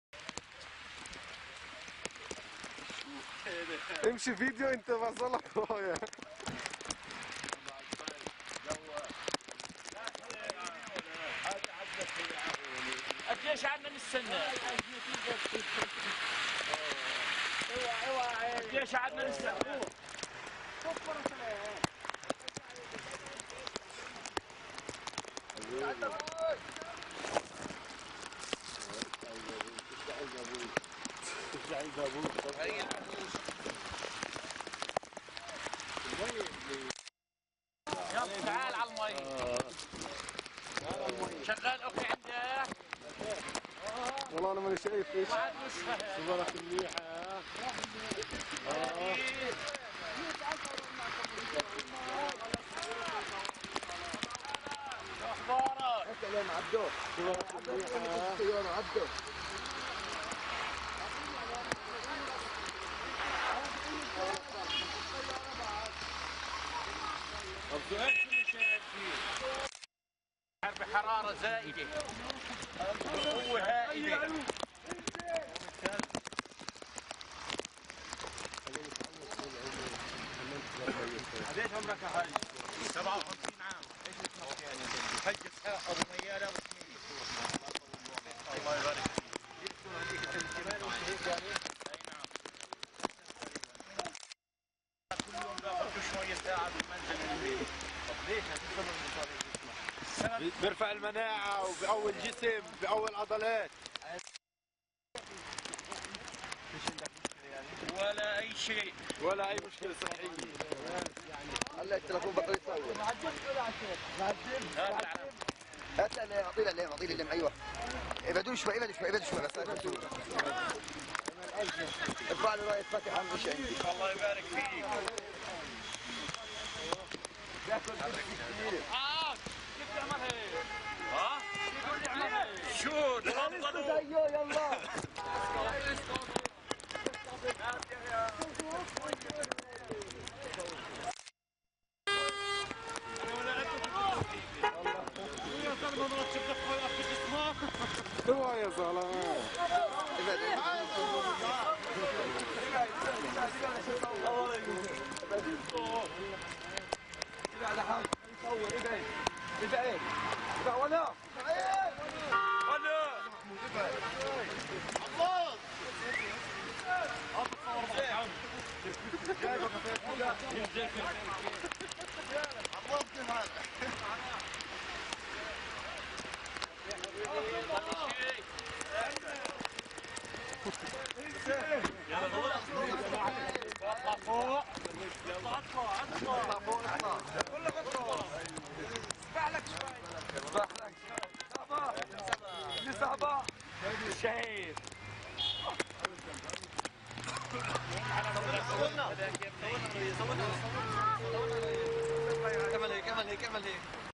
An elderly Palestinian defies the snow
Palestinian man lover of snow inadvertent severe cold, he decided to bathe in a pool of water, half frozen in the city of Hebron.